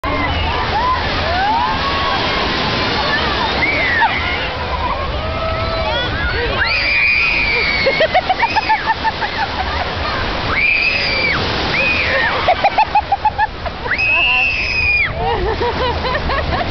Little Kid At Beach.AVI
At the beach with Cortney and Logan in Coney Island